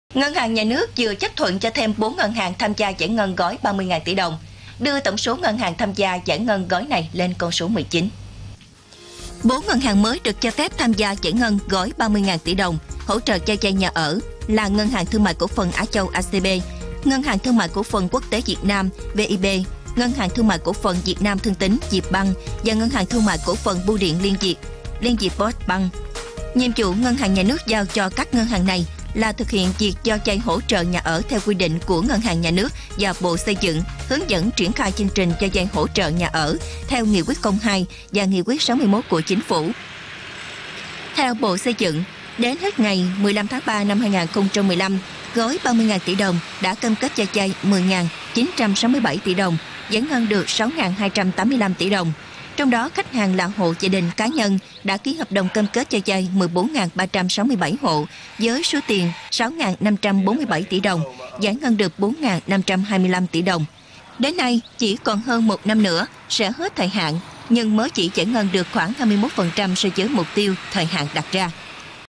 Thêm 4 ngân hàng được giải ngân gói 30.000 tỷ | LATV
LATV | Ngân hàng Nhà nước Việt Nam vừa chấp thuận thêm 4 ngân hàng được giải ngân gói 30.000 tỷ đồng, nâng số lượng lên 19 ngân hàng.

* Điện thoại: ( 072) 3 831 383